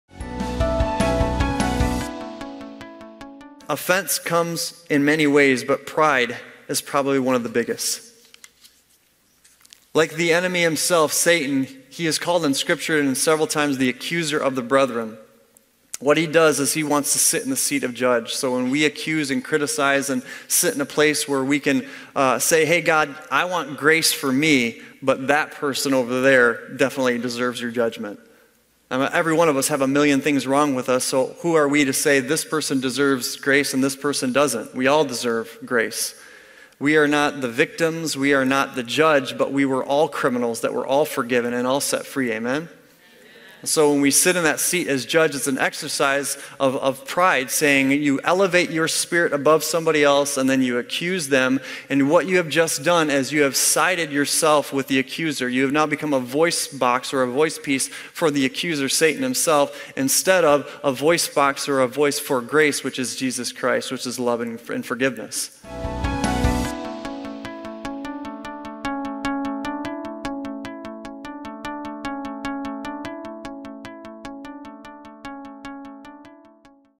Mount Hope Church - Be A Voice For Grace
We can either sit in the seat as judge and partner with the enemy, or we can partner with heaven and extend grace.

Pastor Kevin Berry
Mount Hope Church in Lansing, MI